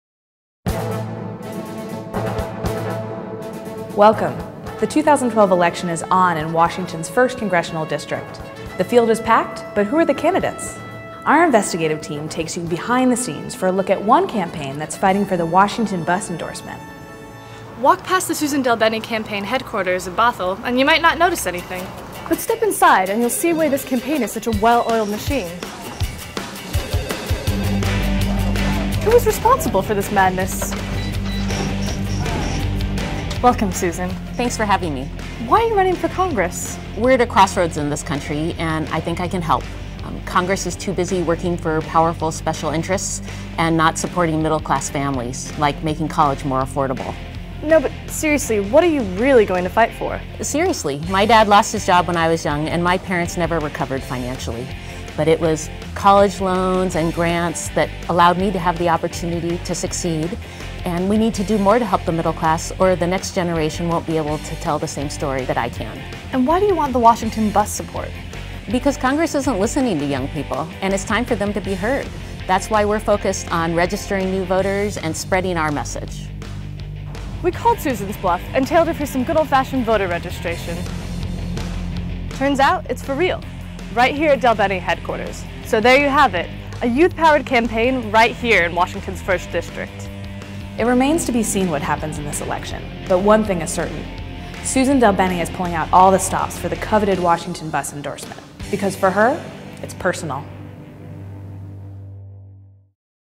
"Breaking News" about Suzan DelBene's efforts to seek Washington BUS endorsement
"News story" about Suzan DelBene's efforts to seek Washington BUS endorsement.